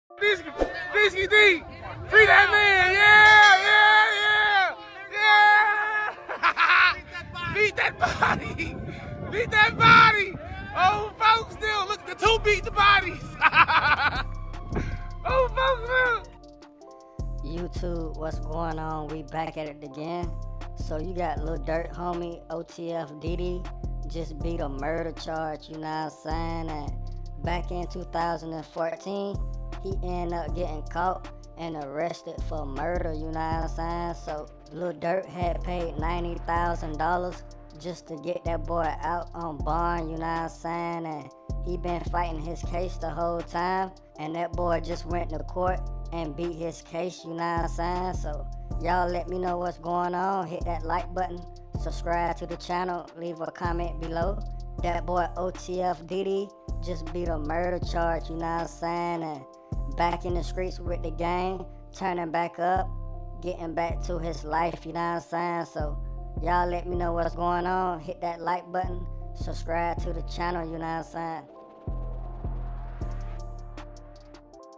Lil Durk Homie OTF Dede Beat Murder Charges After Being Found Not Guilty 🙏😳
Stay Updated with the latest Music,News,Fashion,Games,Cars,Videos,Sports,InstagramLives & More